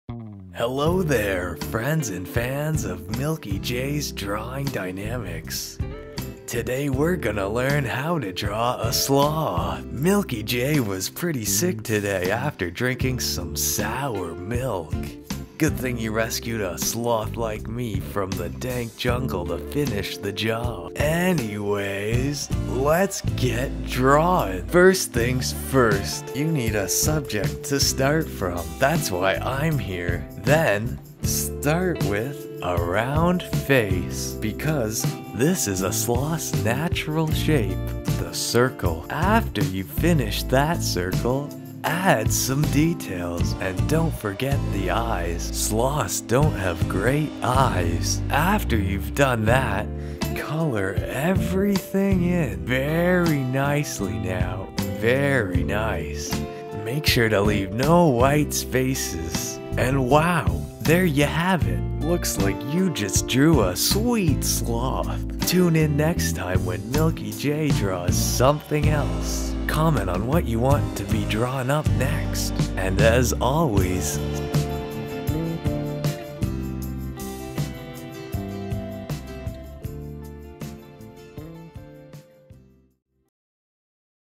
How To Draw A Sloth - Drawing Dynamics - EP4 SE2
Hey there stranger! This time there was an endangered guest taking over the show for MilkyJ as he drank some sour milk making him unable to finish the job. This slothy stranger does his job just fine as he walks us thru the drawing of a sloth. Wow! would you ever guess it!? A drawing lesson from a sloth! What a time we live in! Good old 2015. But anyways, I hope you enjoy the big number 4! Leave a Comment for what you what drawn up next!